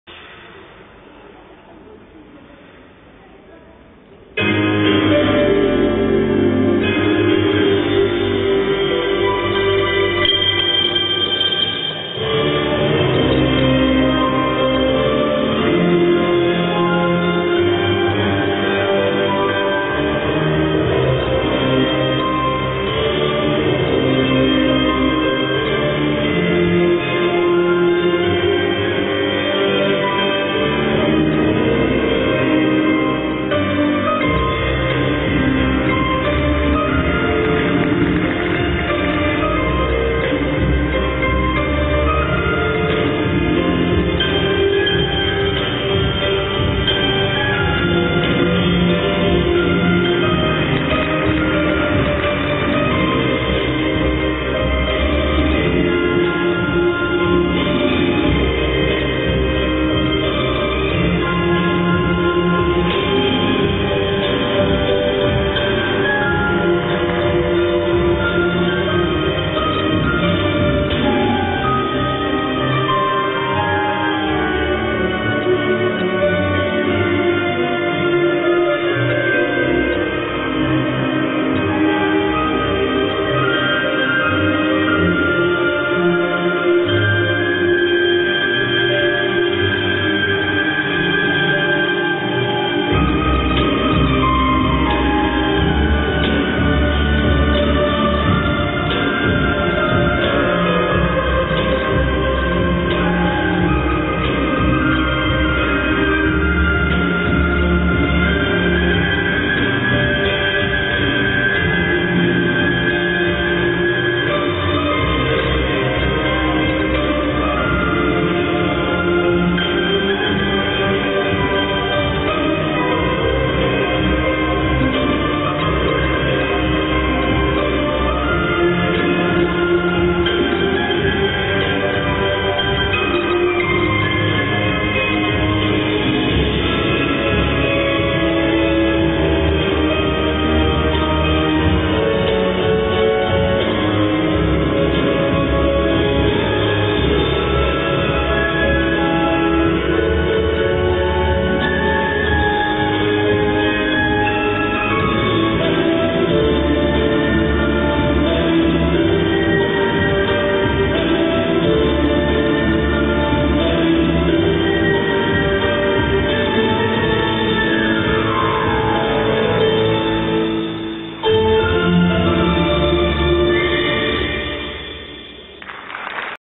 4 M. KLATKA / R. CHRUSCINSKI (POL) - ISU JGP Baltic Cup 2011 Junior Pairs Short Program
To license ISU footage 4 M. KLATKA / R. CHRUSCINSKI (POL) - ISU JGP Baltic Cup 2011 Junior Pairs Short Program